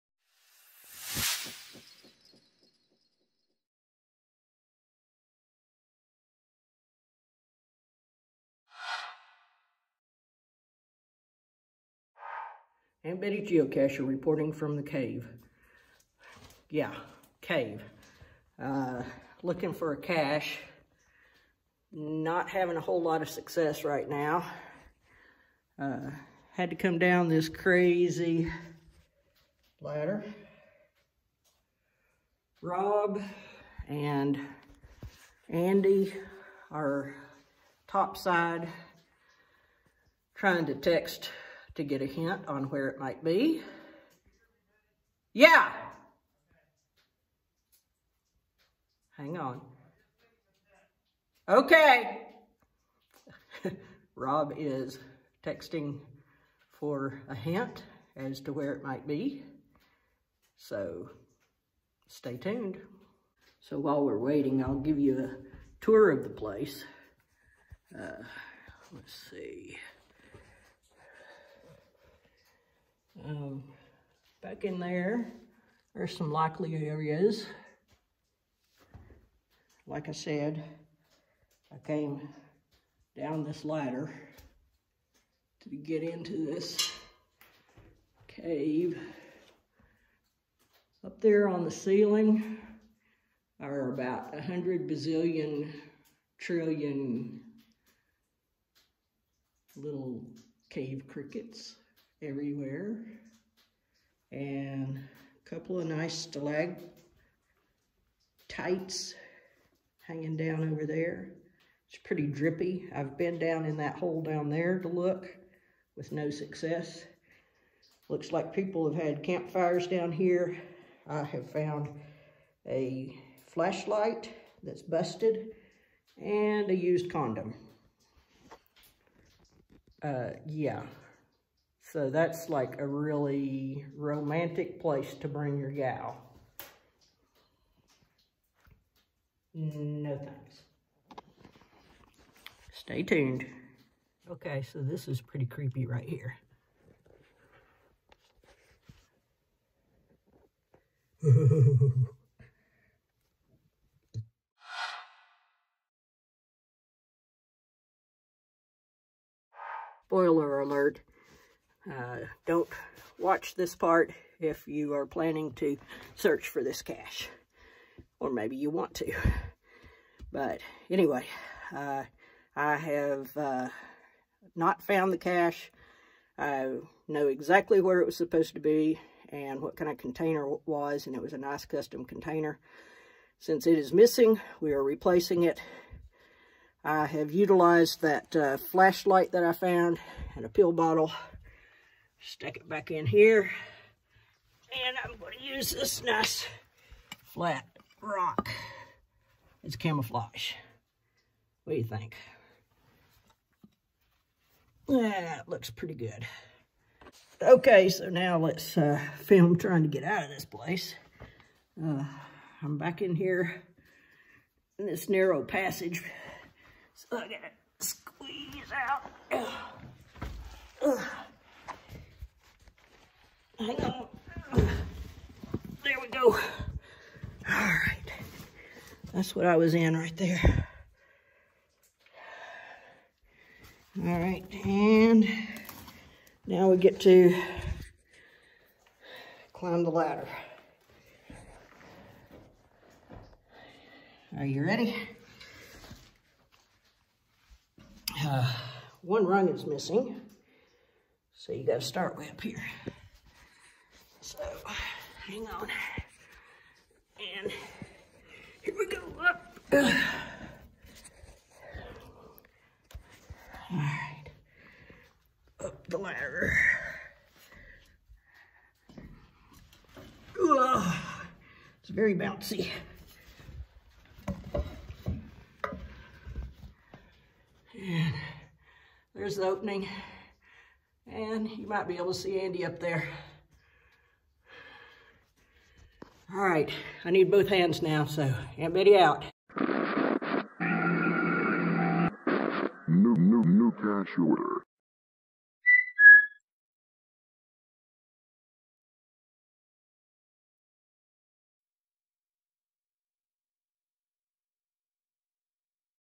antbedy’s adventures: December 15, 2019
Geocaching in a cave - location undisclosed.